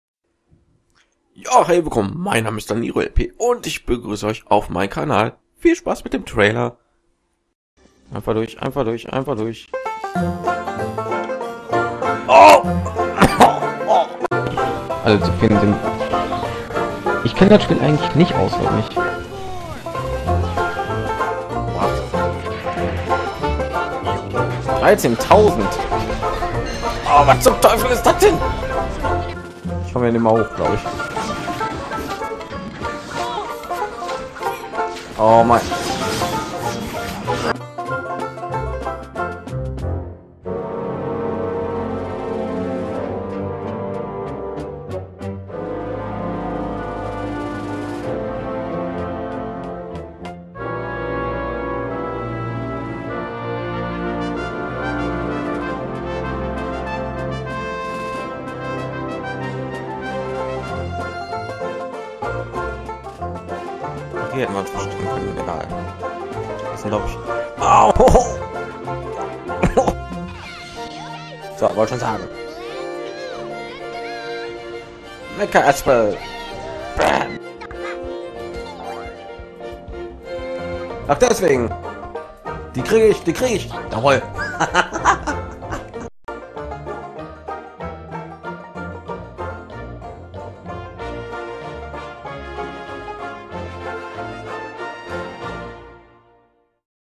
DanieruLP Let's Play Kanaltrailer
Für all die Leute, die sich auf meinem Kanal verirrt haben ist hier ein kleiner Vorgeschmack auf das was sie erwartet.
Viel Spaß ^^

DanieruLP Kanal-Trailer

Wenn euch verbesserungen einfallen,ihr komplimente ablassen wollt oder mir einfach nur sagen wollt das ich Schlecht bin,der kommentar bereich steht euch ruhig offen ^^.
Über Feedback würde ich mich sehr freuen und schonmal im vorraus: Danke für das schauen meines Videos ^^